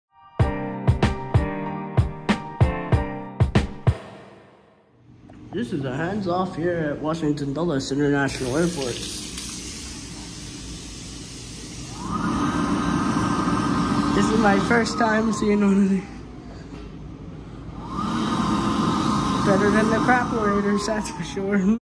Excel Dryer Hands Off At IAD concourse E
date taken October 10th 2022 waiting for my flight to Montreal Quebec